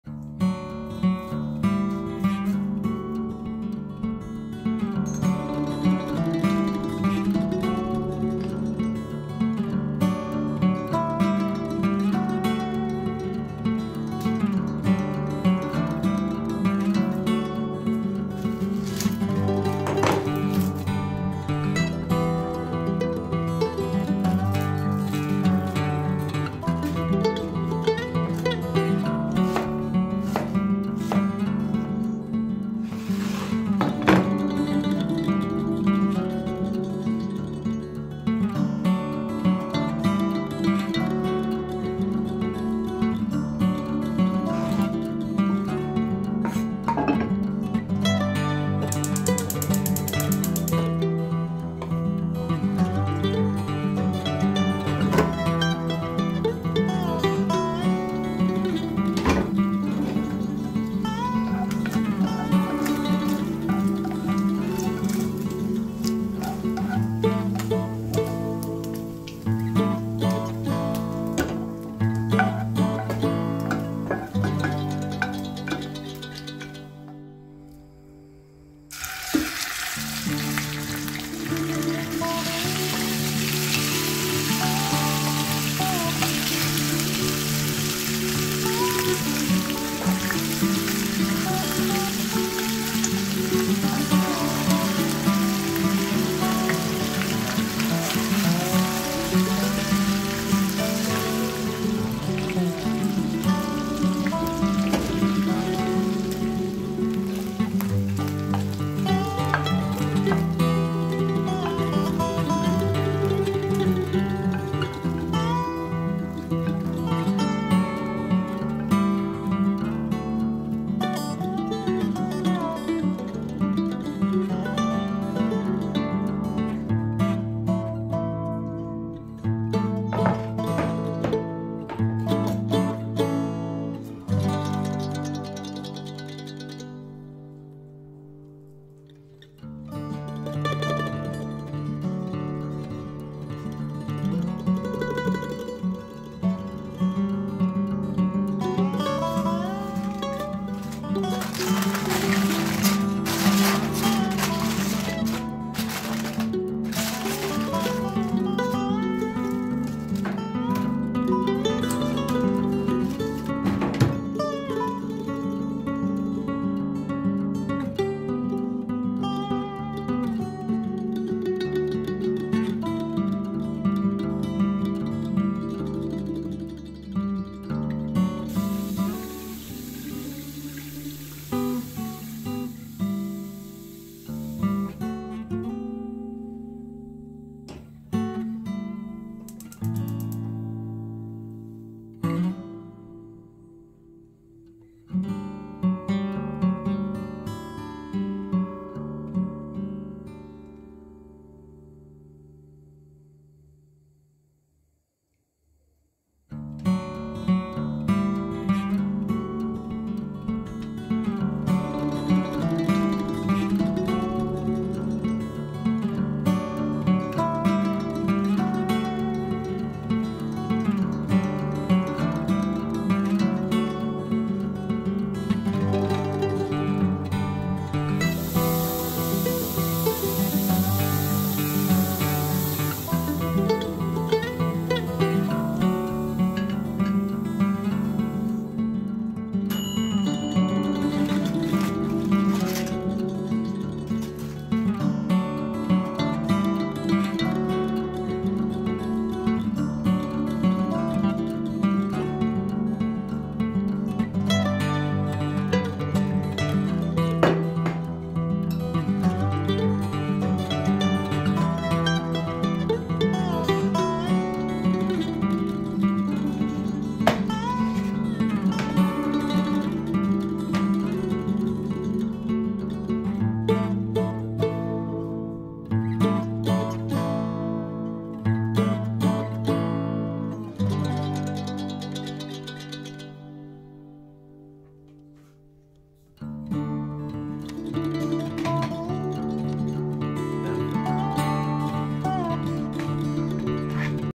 Want to eat lamb with rice? Try this recipe
Lamb meat feed.With dill rice
the most delicious dishes you can cook at home!!
Lamb meat with dill rice is a delicious and popular dish that is used in Iran and other parts of the world.
This food has a very delicious and fragrant taste and is usually combined with garlic.
 In addition, other spices such as turmeric, black pepper and salt are also used to give the food a unique taste.
ingredients:
Lamb meat(600gr)
Rice (300gr)
onion (3)
garlic (1)
dry dill (2sp)
spices (saffron, salt, black pepper, red pepper, turmeric)